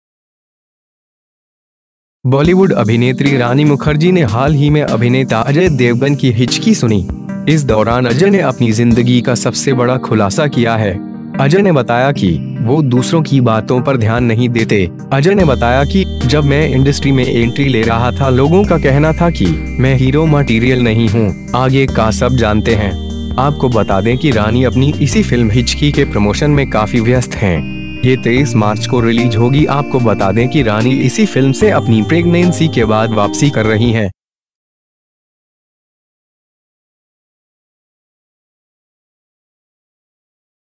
अभिनेत्री ने रानी मुखर्जी ने सुनी अजय देवगन की हिचकी, सुने क्या कहा अजय देवगन ने
बॉलीवुड अभिनेत्री रानी मुखर्जी ने हाल ही में अभिनेता अजय देवगन की हिचकी सुनी। इस दौरान अजय ने अपनी जिंदगी का सबसे बड़ा खुलासा किया है।

अजय ने बताया कि, वो दूसरों की बातों पर ध्यान नहीं देते, अजय ने बताया कि, जब मैं इंडस्ट्री में एंट्री ले रहा था लोगों का कहना था कि, मैं हीरो मटीरियल नहीं हूं, आगे का सब जानते हैं। आपको बता दें कि रानी अपनी इसी फिल्म हिचकी के प्रमोशन में काफी व्यस्त हैं। ये ​23 मार्च को रिलीज होगी आपको बता दें कि रानी इसी फिल्म से अपनी प्रेग्नेंसी के बाद वापसी कर रही हैं।